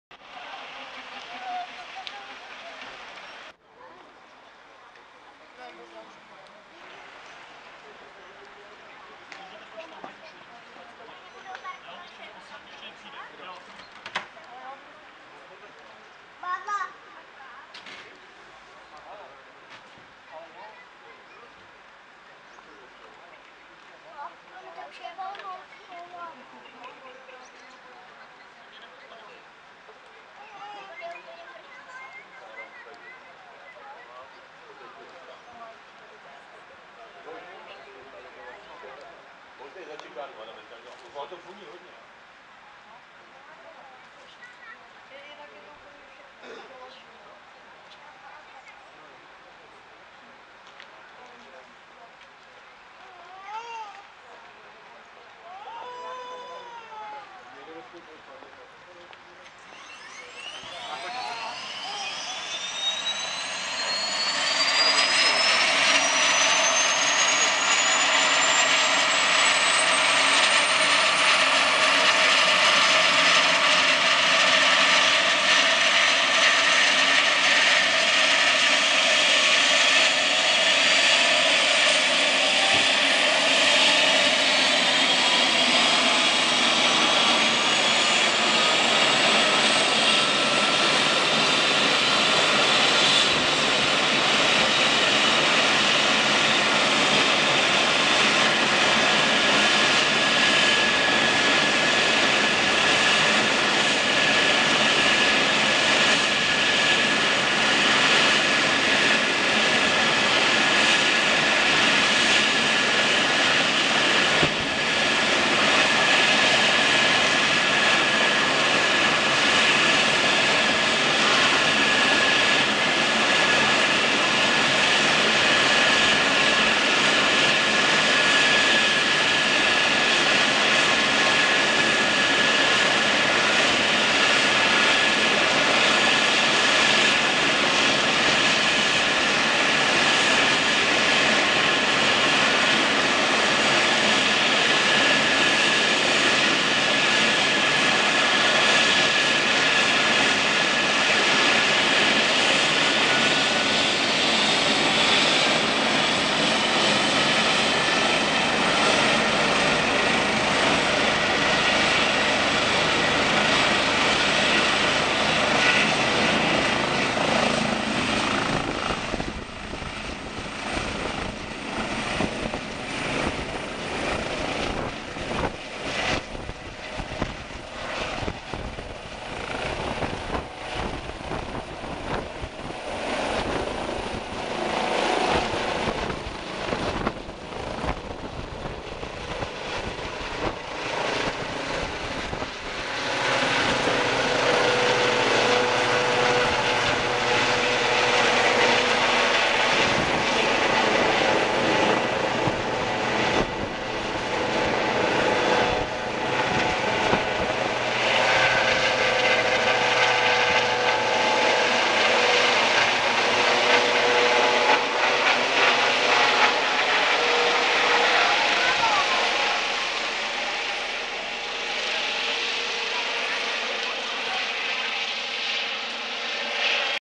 PELHŘIMOV: Záchranářský vrtulník v Pelhřimově
Letecká záchranná služba. Odlet zasahujícího vrtulníku na sídlišti Táborská v Pelhřimově. Středa 9. srpna 2017 po půl osmé večer.